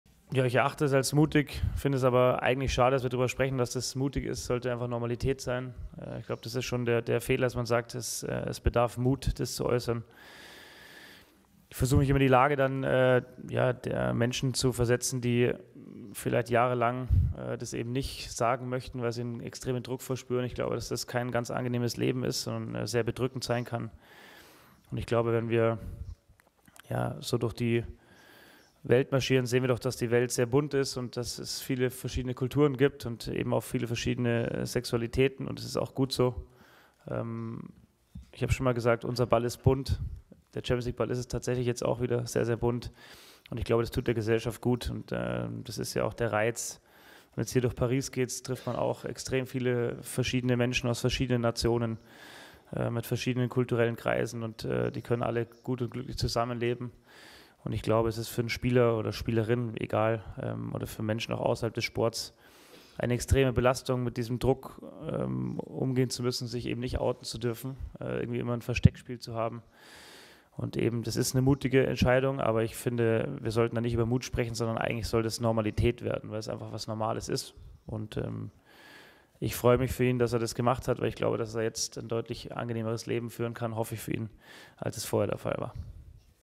"Unser Ball ist bunt!" - Nagelsmann mit starken Worten zum Outing von Jakub Jankto | FC Bayern
Vor dem Hinspiel im Achtelfinale der UEFA Champions League richtet Julian Nagelsmann, Trainer des FC Bayern München, auf der Pressekonferenz ein paar Worte der Unterstützung an Jakub Jankto. Jankto hatte sich wenige Stunden zuvor als homosexuell geoutet.